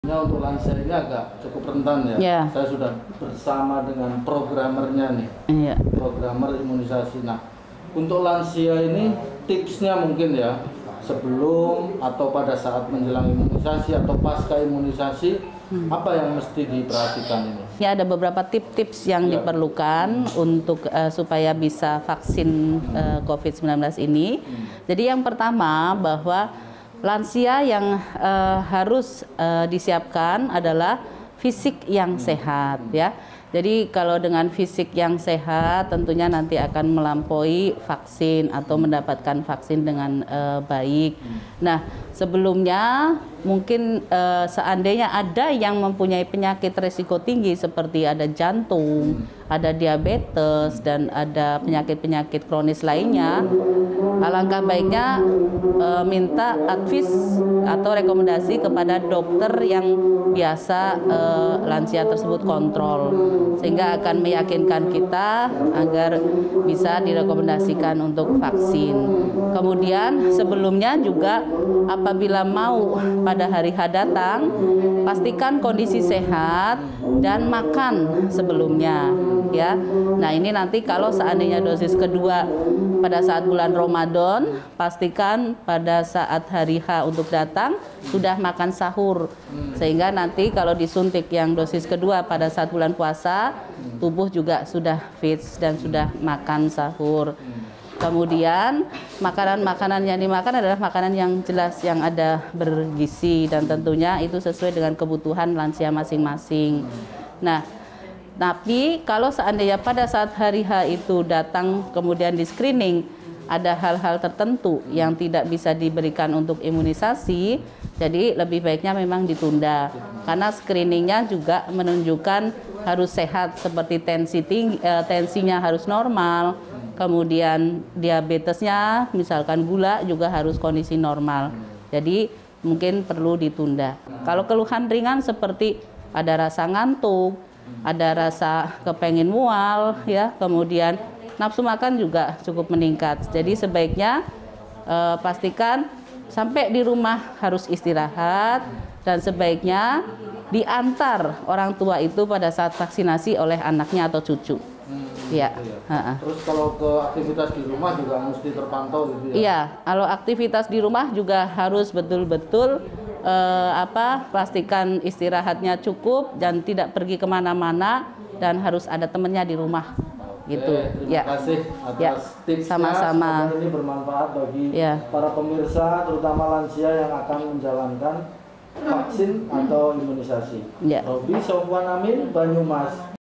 Tips Vaksinasi Covid 19 Untuk Lansia
Demi mencegah penyebaran Covid-19, sejumlah lansia di Banyumas, Jawa Tengah, sudah menjalani vaksinasi.

Lalu, bagaimana prosesnya dan tipsnya membawa orang tua menjalani vaksinasi? Berikut Liputannya